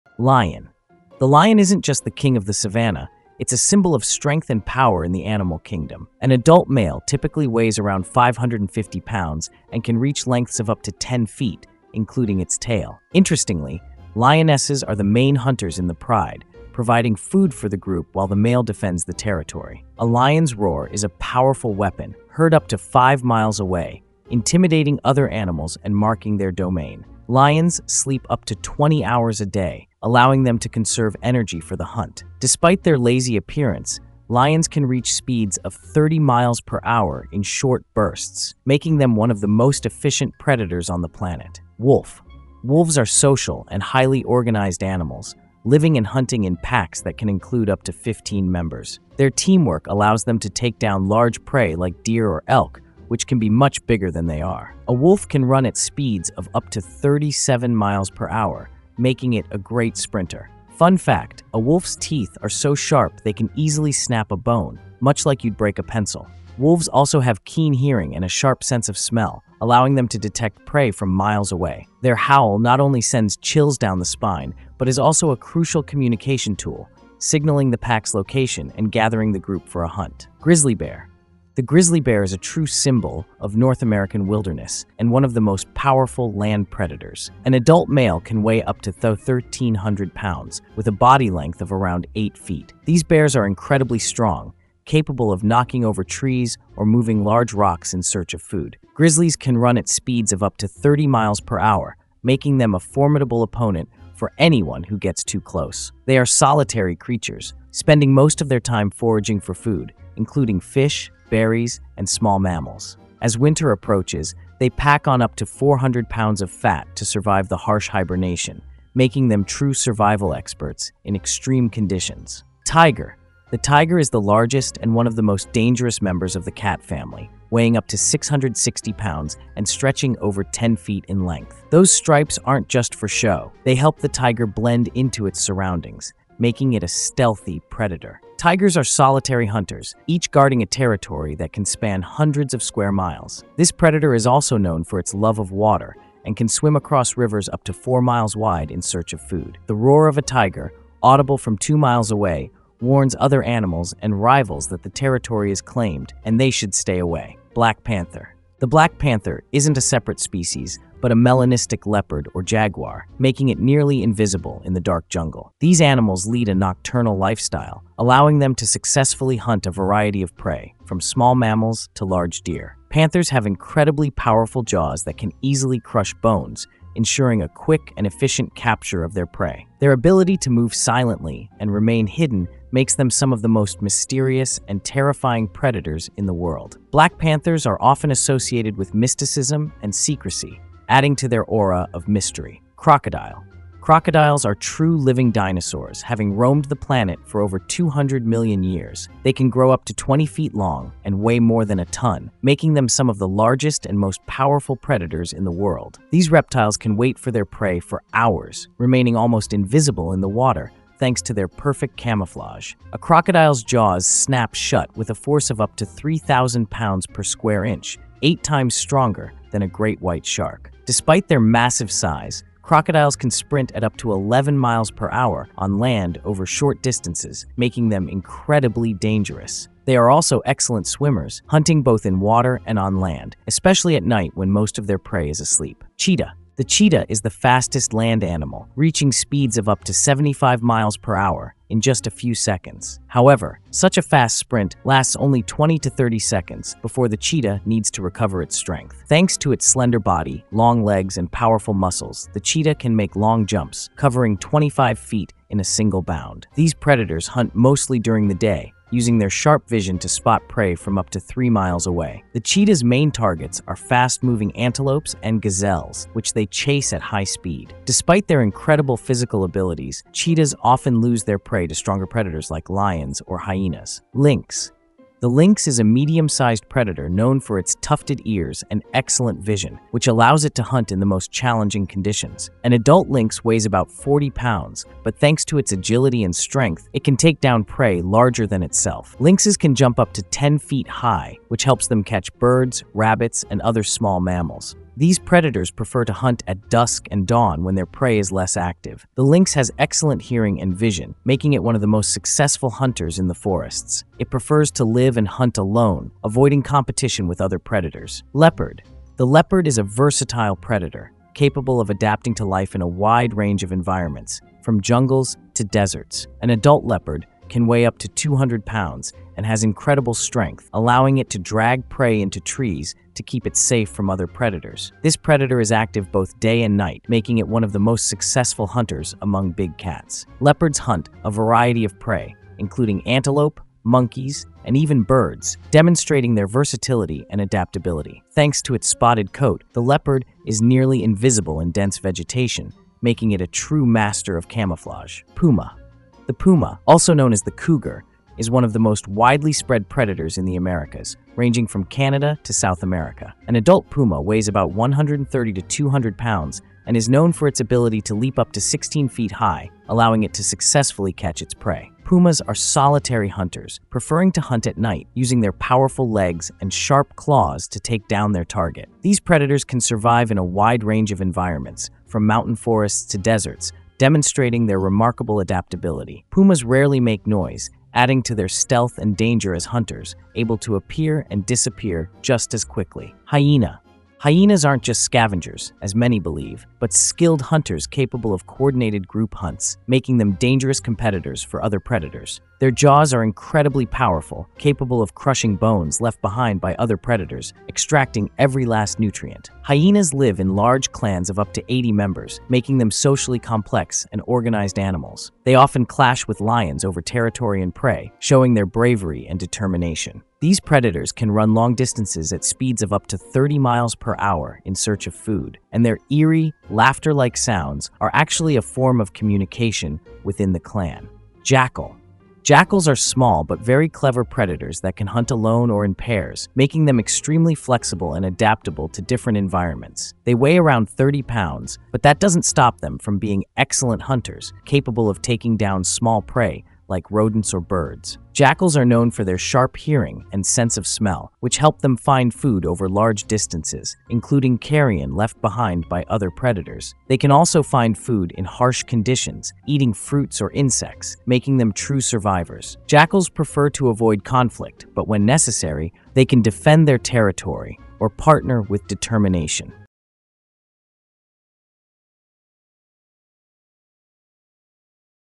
Every Land Predator Explained in 7 minutes
Welcome to the exciting world of land predators! In this exciting video, you'll be immersed in a world of wildlife where savannah kings, and desert hunters battle for survival.
From mighty lions and fast cheetahs to cunning bobcats and indomitable bears, each predator has its own unique hunting strategies and adaptations.
Prepare for an exciting journey into the world of land predators where strength, agility and instincts play a crucial role in the fight for survival.